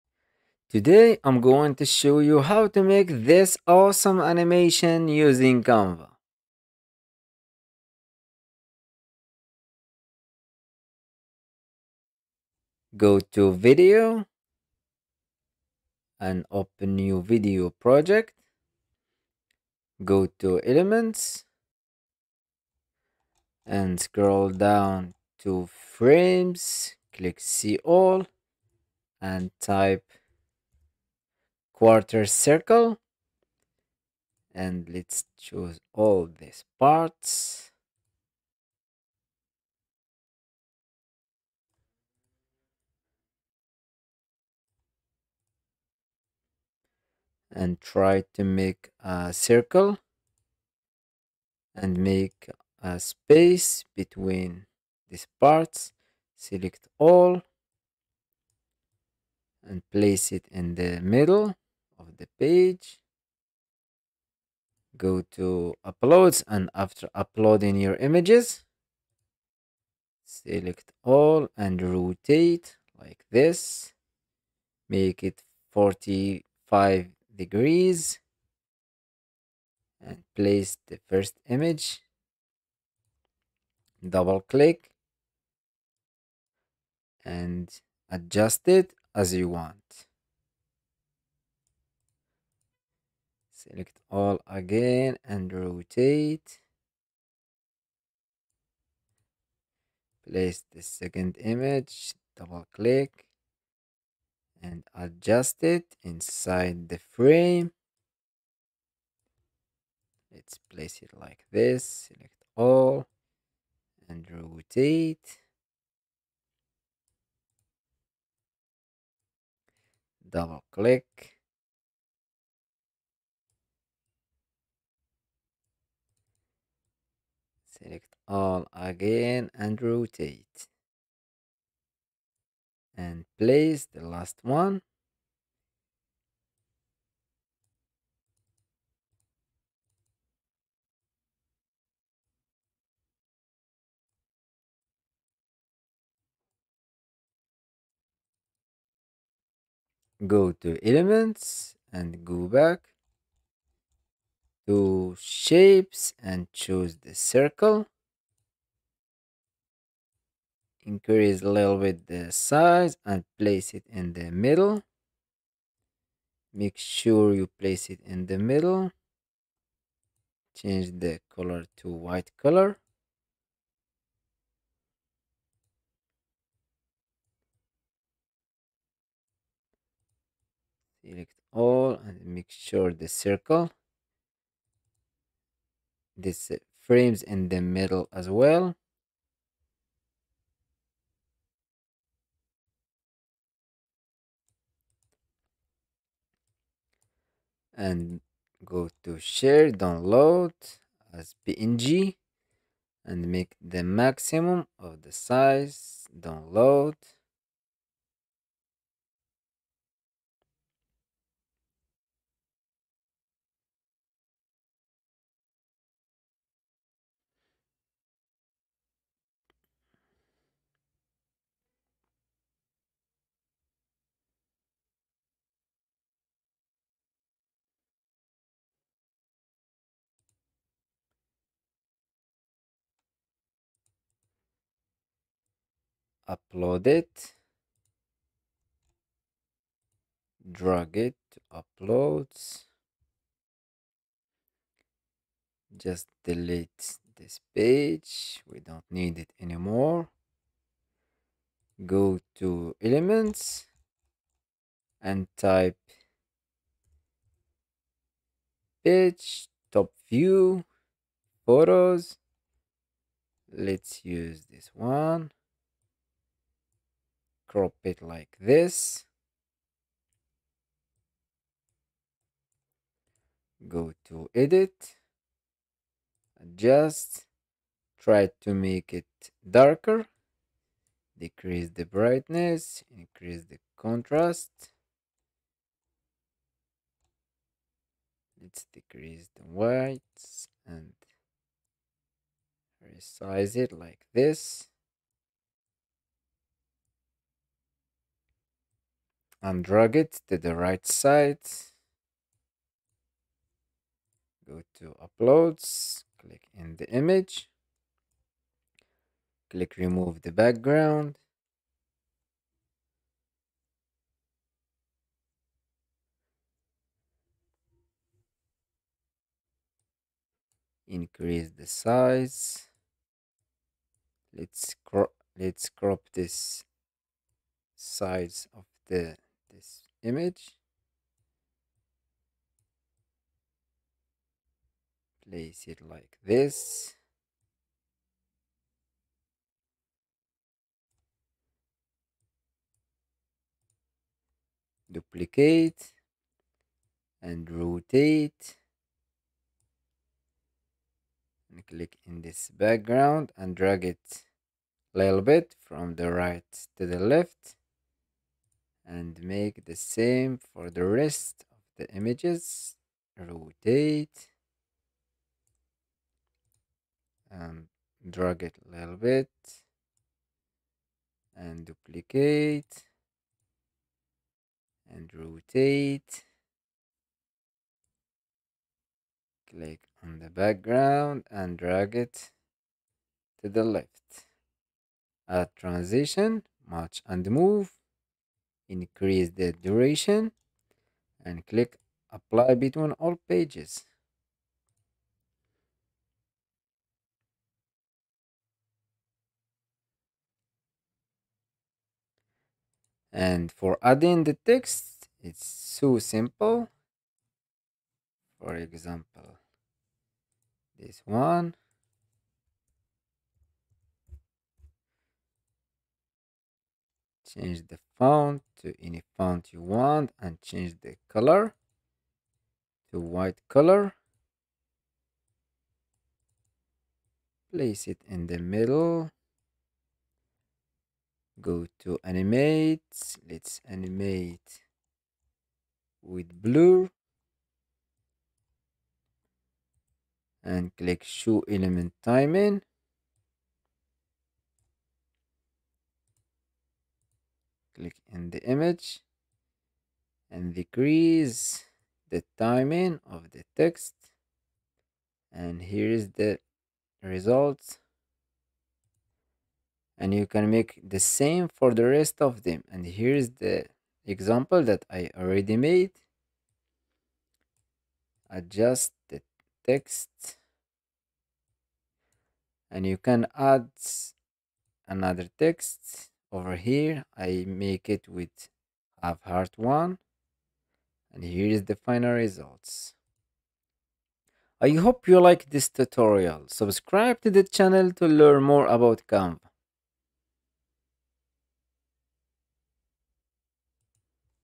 Rotate Like a Pro in Canva Elevate Your Presentations with Ease!
Master the art of rotating elements in Canva like a pro! 🚀 In this tutorial, I'll show you how to rotate text, images, and shapes perfectly to create stunning designs and elevate your presentations. Whether you're working on slides, social media posts, or any creative project, these tips will take your Canva skills to the next level!